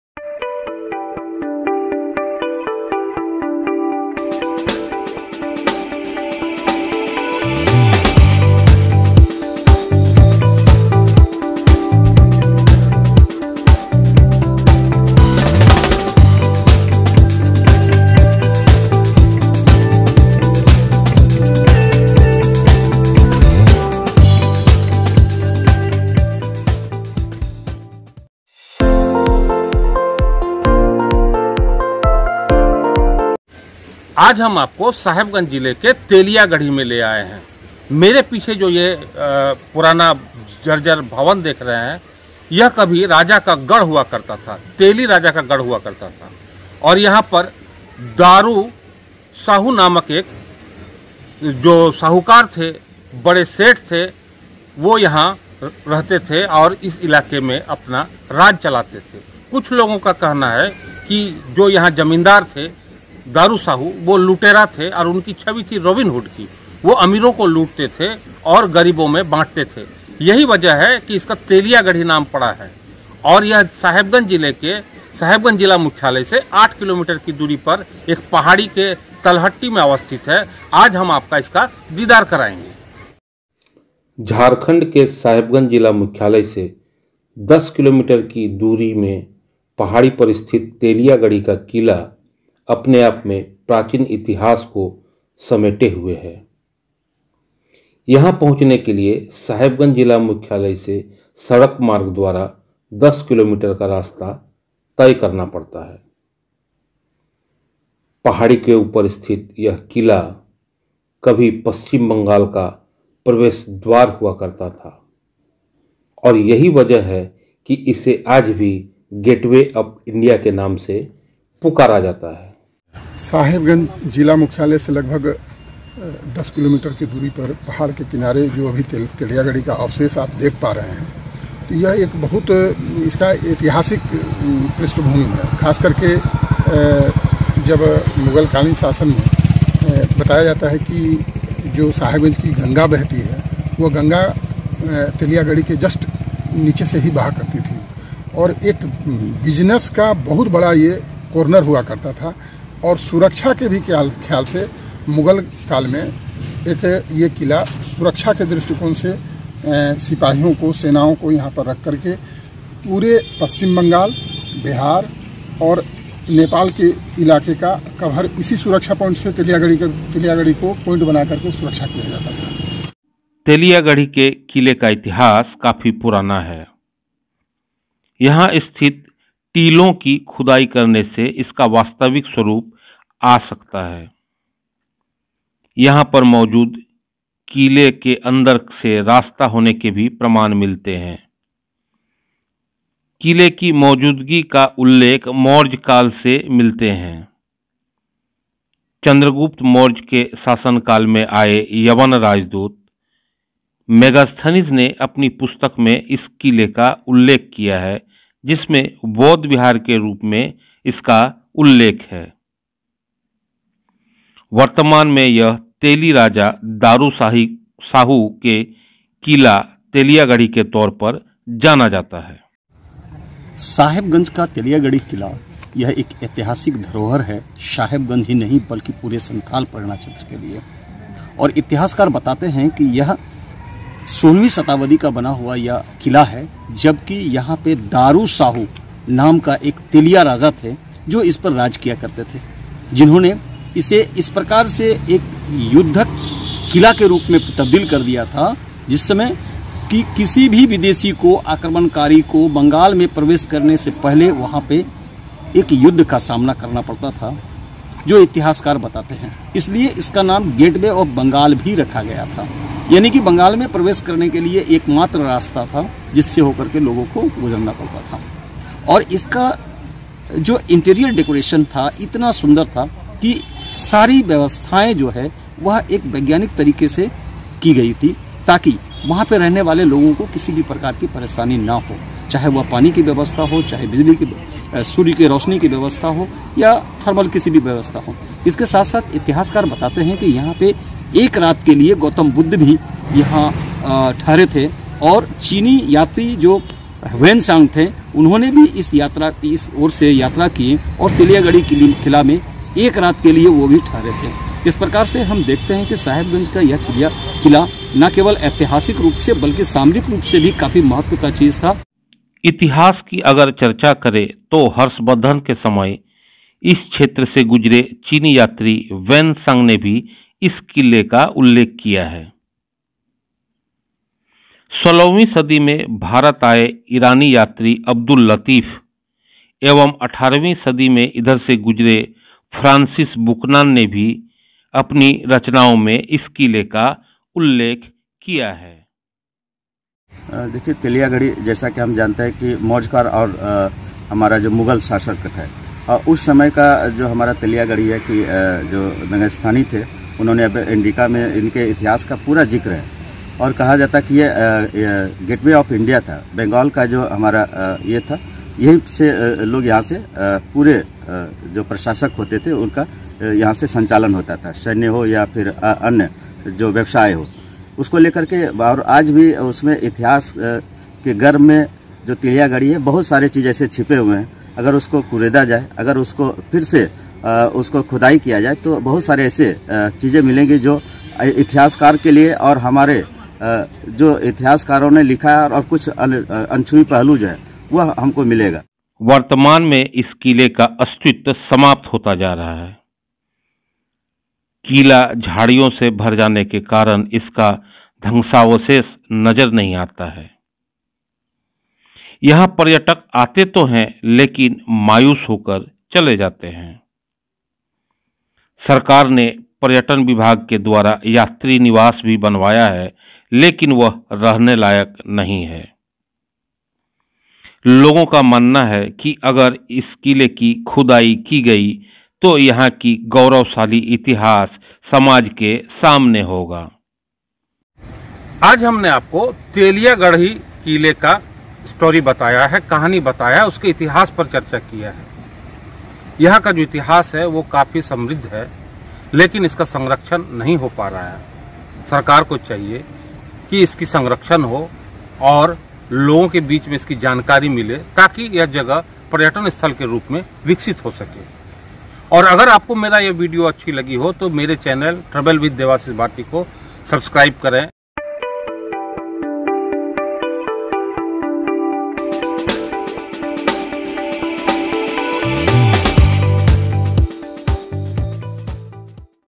तेलियागढ़ी का किला || TELIYAGHADHI FORT || SAHEBGANJ
Teliagarhi became the main gateway for Muslim armies coming from northern India and marching to Bengal from the 13th century. Bakhtiyar Khilji passed through Teliagarhi pass on the way to the conquest of Bengal. In 1538, Sher Shah Suri and Humayun fought a decisive battle near Teliagarhi. The rebellious Prince Khurram fought with Ibrahim Khan at Teliagarhi and Rajmahal for control of Bengal.
Teliagarhi fort was built by a Teli zamindar who converted to Islam during the rule of Shah Jahan. Maa Raksisthan temple was built near the fort in 1819.
source- Wikipedia

🤵 Debashish Bharati social Handles:-

For Businesses related queries mail